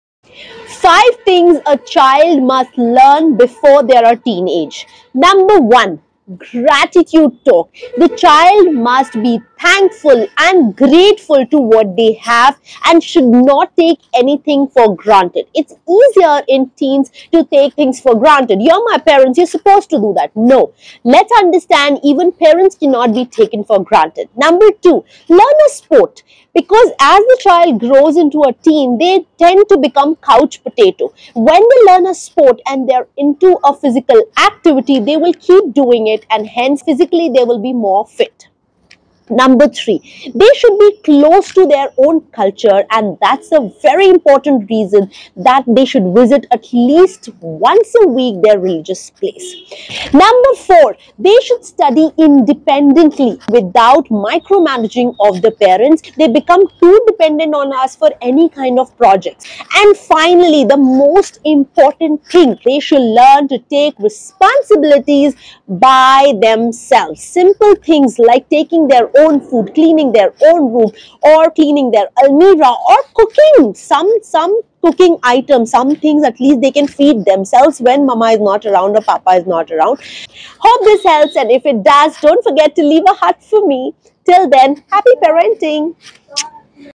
5 things the child must know and must do before teenage
5 things the child must know and must do before teenage .
Let me know your thoughts on this?
If you have any point which feel I have missed please mention in the comment box 📦 🙂

Follow @parenting_swati for more such tips..
Share with friends and family who have kids more than 5 years of age ❤️💞

Cheers
Swati